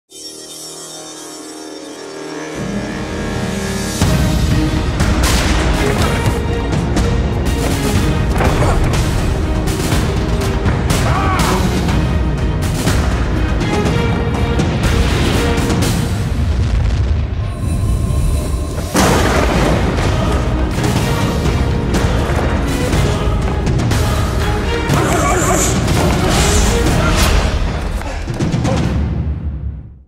Lord of the Takedown Commercial
My latest project is a feature film on former UFC champ Evan Tanner.